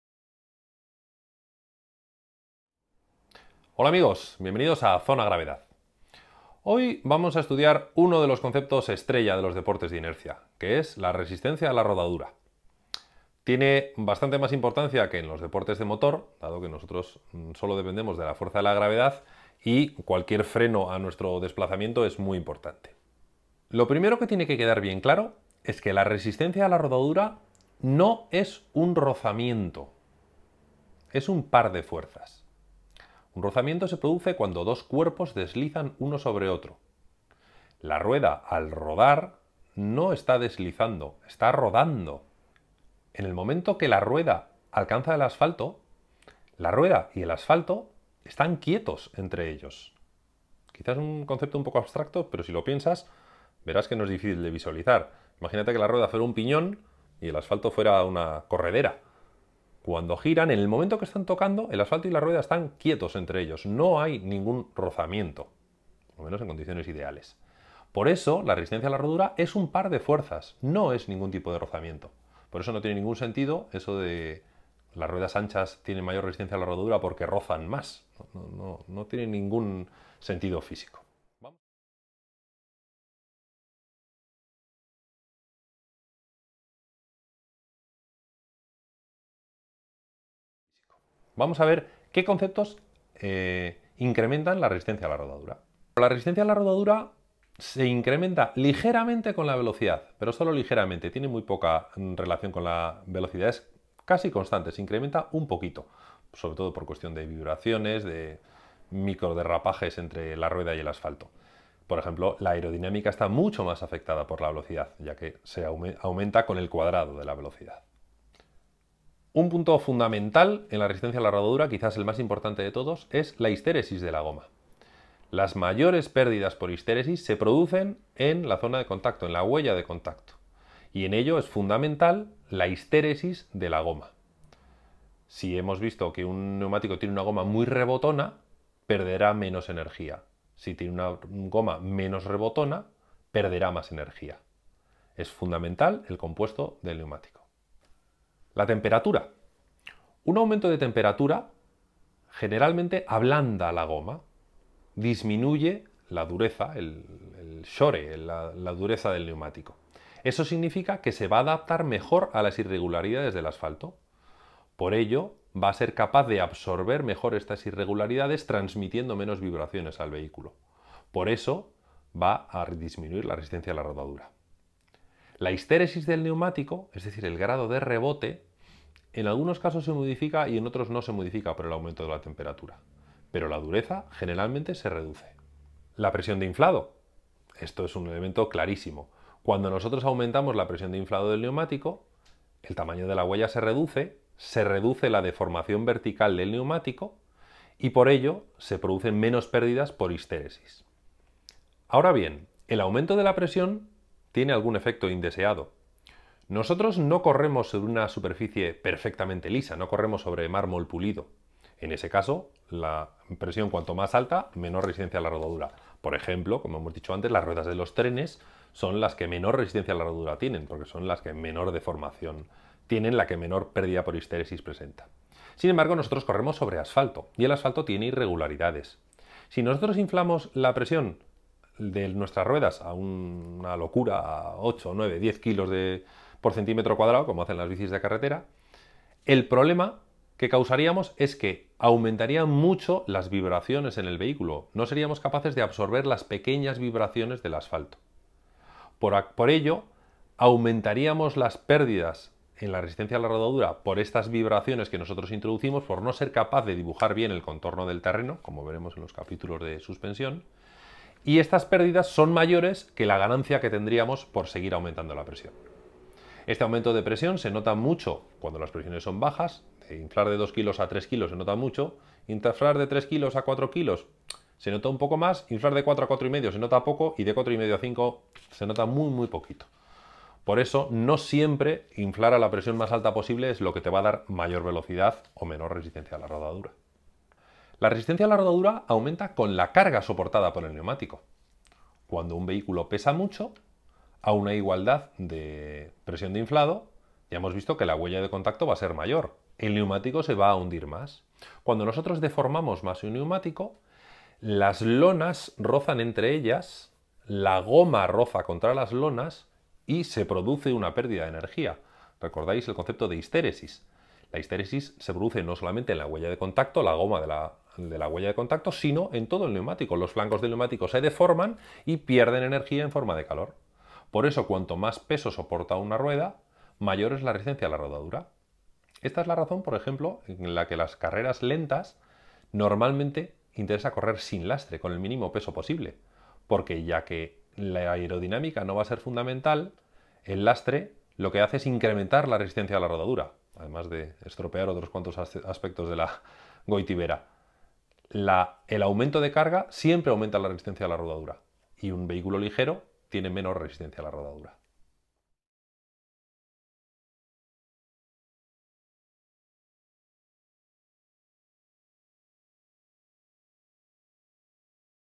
Dinámica 11: Resistencia a la rodadura ( I )
Qué factores influyen en la resistencia a la rodadura de nuestros neumáticos ( 1ª parte )